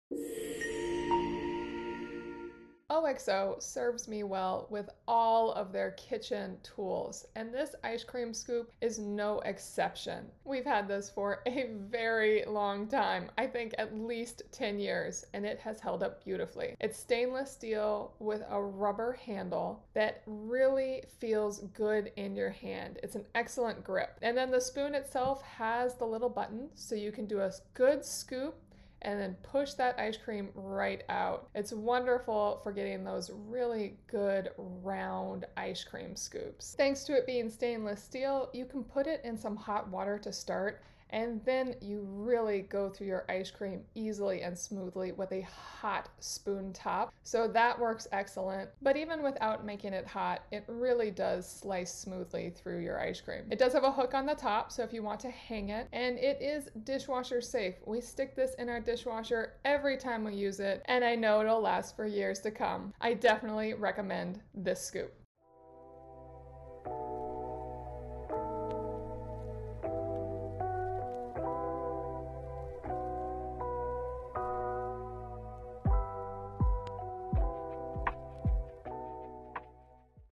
No More Bent Ice Cream Scoops - Try the Heavy Duty OXO Ice Cream Scoop For Perfect Servings!
Enjoy Perfect Scoops Every Time with the OXO Ice Cream Scoop (Amazon)

Tired of struggling to scoop ice cream with a bent scoop? Look no further than the OXO Ice Cream Scoop. This stainless steel scoop is designed with a pop-up lever that makes it easy to scoop and release ice cream. We've had this for years and it has held up beautifully, giving me excellent scoops every time. Though I do recommend heating the OXO Ice Cream Scoop with hot water when you're dealing with extra hard ice cream!

(Amazon)
The newer model looks excellent too (Amazon)